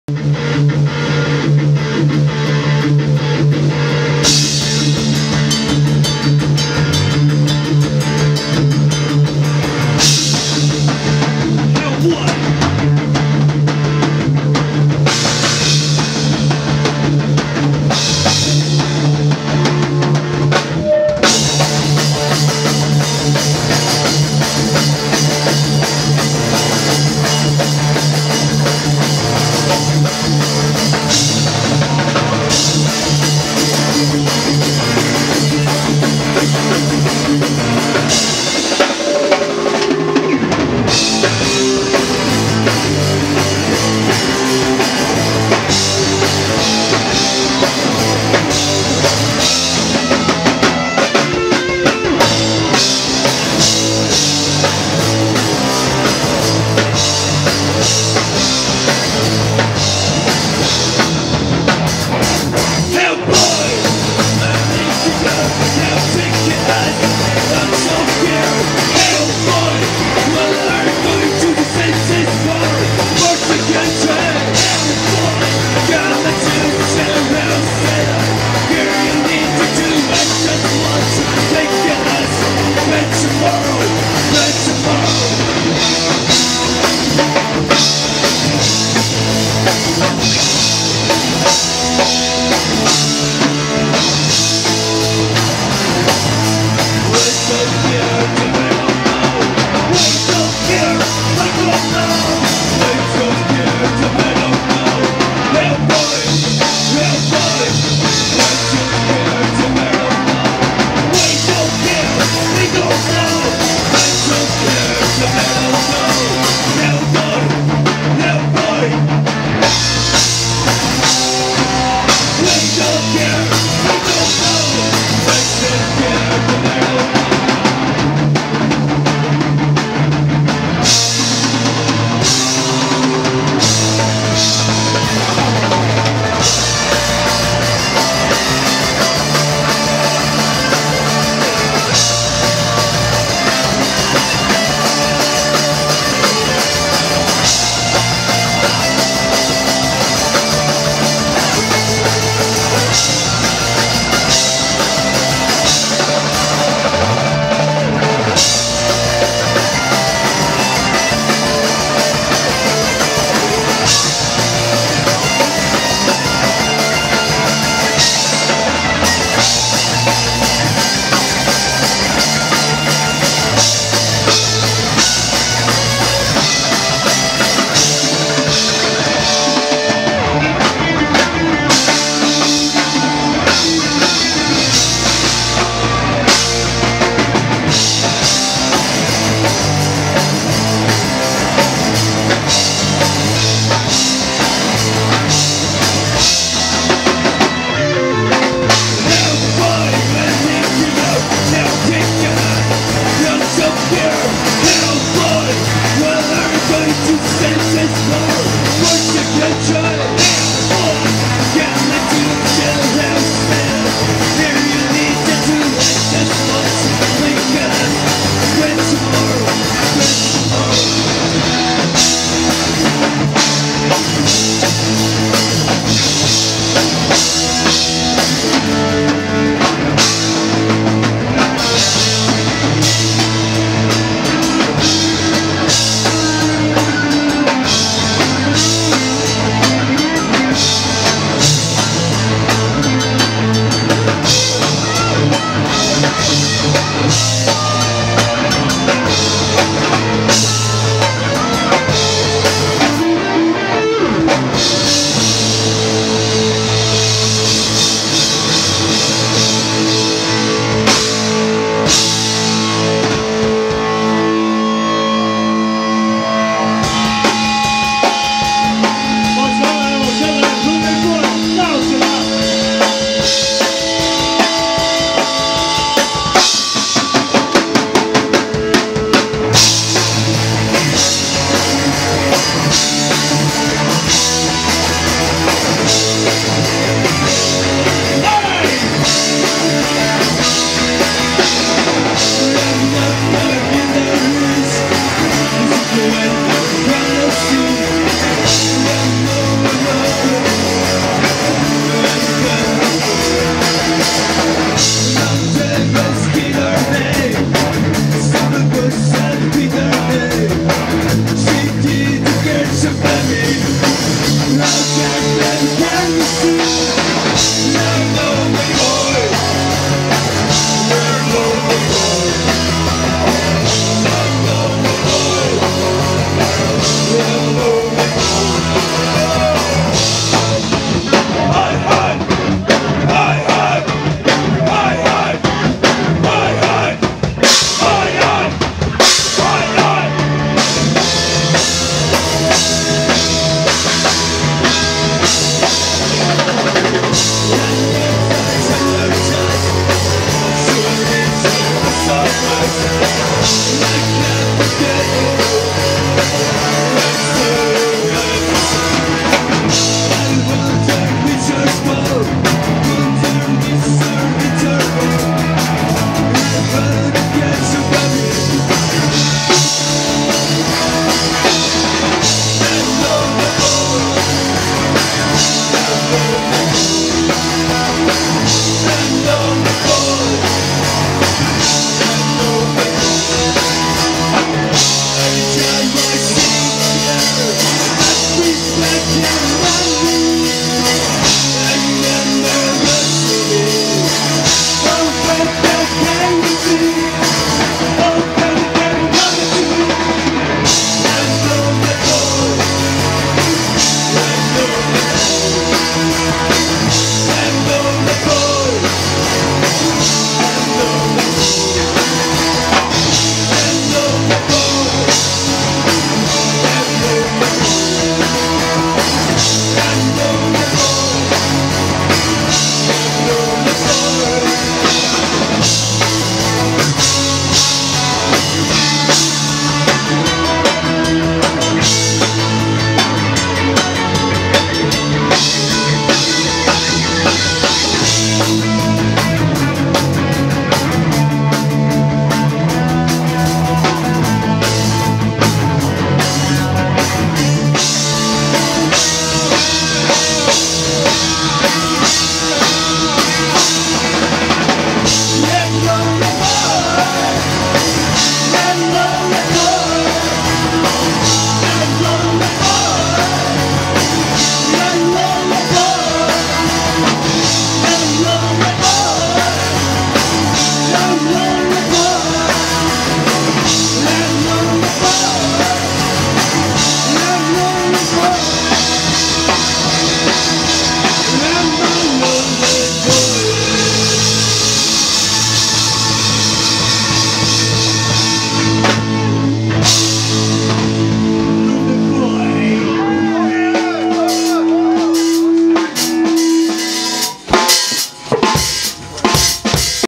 Hell City @ Old What Bar
Beijing punk band Hell City playing at Old What Bar, October 31, 2013.